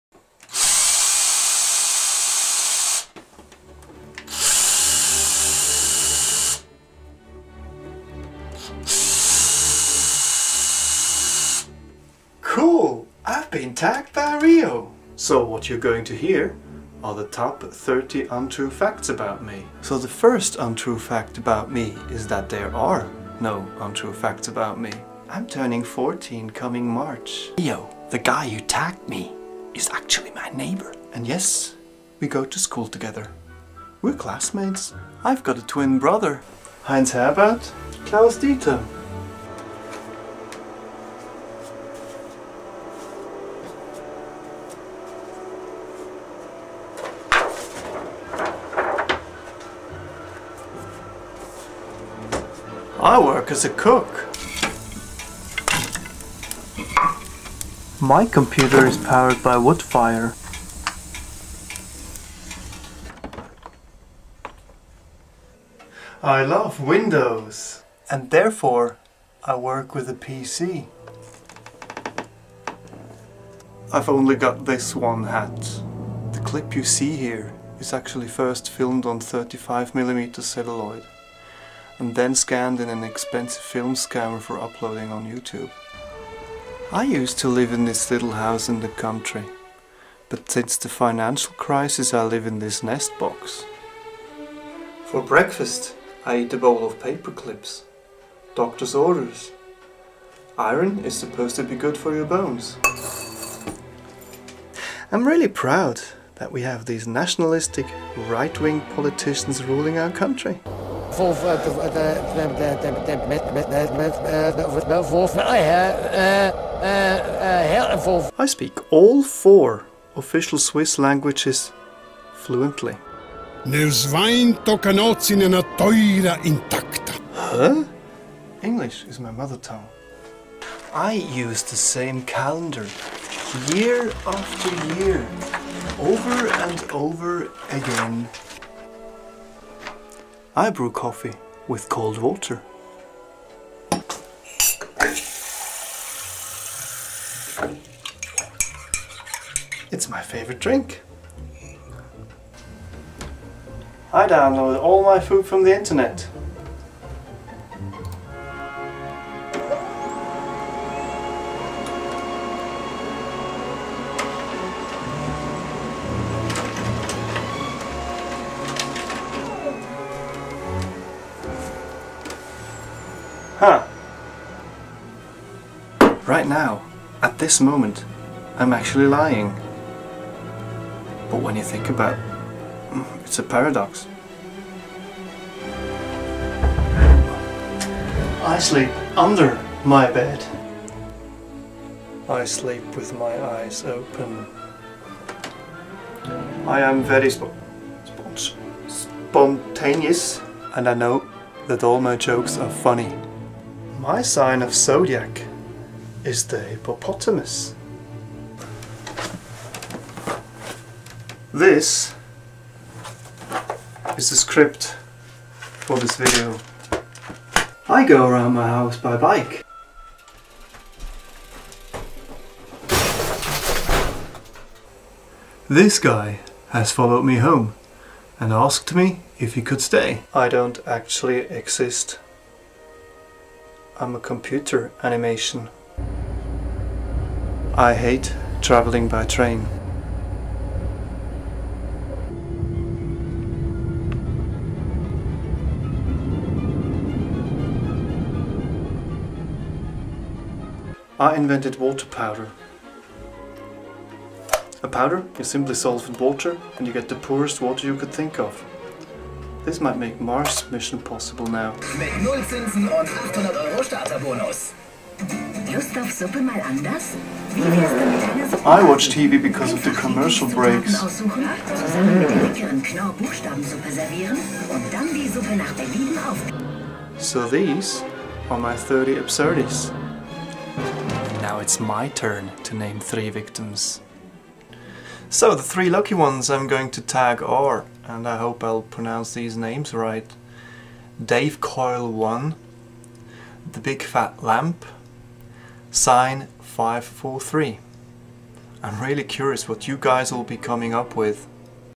My cat does the ironing
I tag...

To make your own 30 Absurdities video:

1. Get Tagged

2. Make up 30 outrageous untrue facts about yourself. And three people to tag

3. Film yourself saying these facts and the people you are taging.

4. Edit into a fast pace

5. Add music

6. Upload to Youtube

7. Send video to who you are taging

Optional create clips to go with the facts